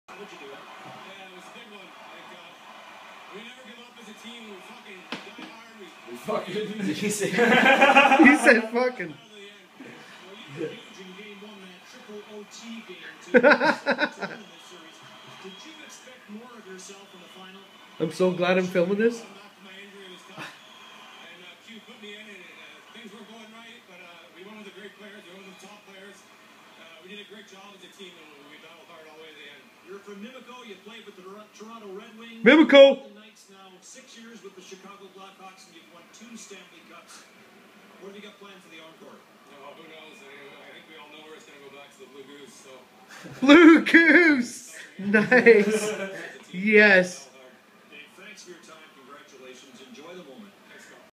Bolland says "fuckin" and blue goose
2013 stanley cup shamps chicago blackhawks dave bolland scores game winner. Says "fuckin" in interview and "takin it back to the goose"